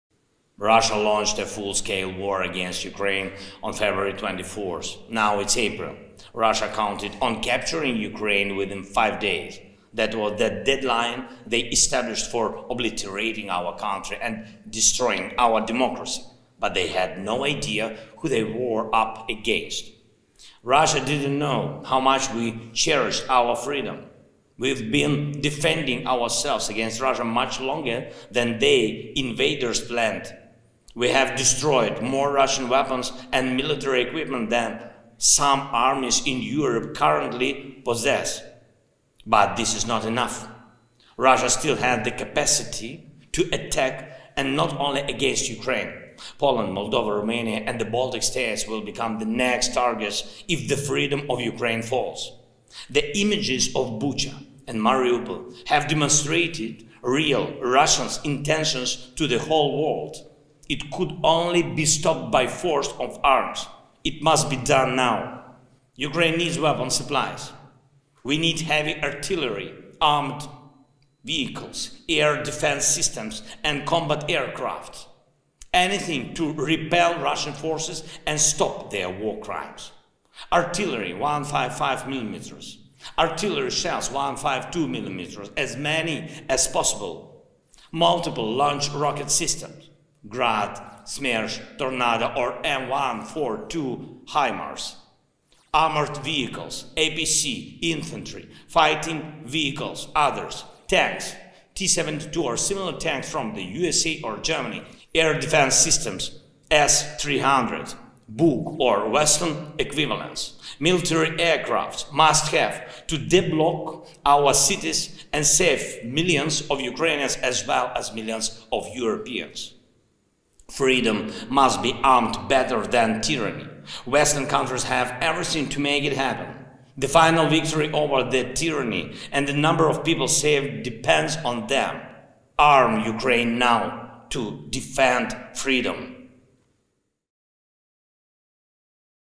Україні потрібне важке озброєння
Щоб продовжувати героїчно захищати світ від російської агресії, Україні потрібен конкретний перелік зброї. Звертаюся до громадян усього світу з проханням допомогти донести до ваших урядів, адміністрацій президентів, керівників ваших країн реальні потреби України, які допоможуть зупинити війну.

Нам терміново потрібна важка артилерія, важка бронетехніка, системи протиповітряної оборони та літаки:
- артилерійські установки (калібр 155 мм) і боєприпаси;
- артилерійські снаряди (калібр 152 мм). Якомога більше;
- реактивні системи залпового вогню: «Град», «Ураган» або американський M142 HIMARS;
- БТР (бронетранспортери, бойові машини піхоти тощо);
- танки (Т-72, американські або німецькі аналоги);
- системи ППО (С-300, «Бук» або подібні сучасні західні системи протиповітряної оборони);
- бойова авіація.

Ми просимо весь світ об’єднатися й допомогти нам у боротьбі з цією жорстокою агресією. Дайте Україні зброю зараз, щоб разом перемогти це зло.